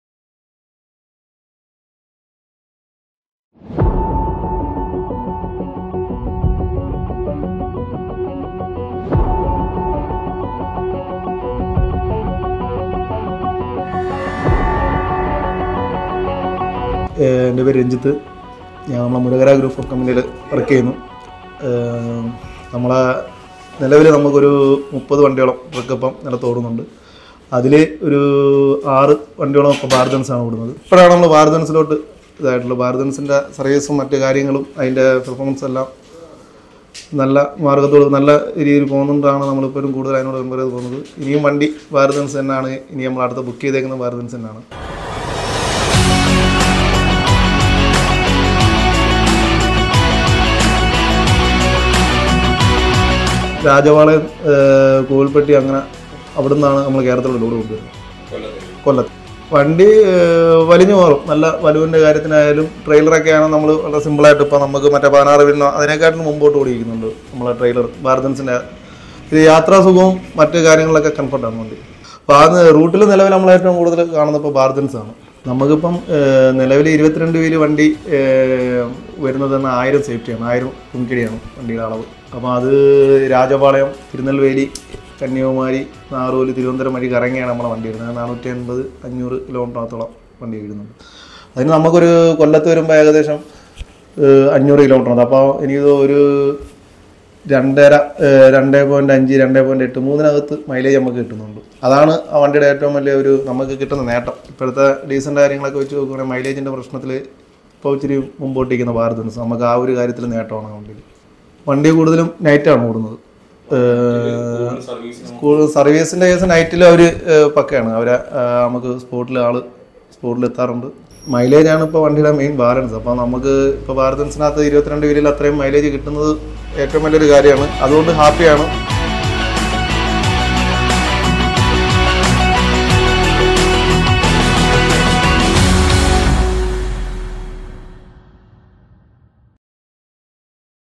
Murahara Group | Autobahn Trucking | BharatBenz Customer Testimonial
We appreciate Mr. Renjith, an employee at Murahara Group for taking the time to talk to us about the experience with Autobahn Trucking.

We at, Autobahn Trucking consider it an honour that Murahara Group have chosen to continue doing business with us, and we are grateful to be able support your company for the foreseeable future.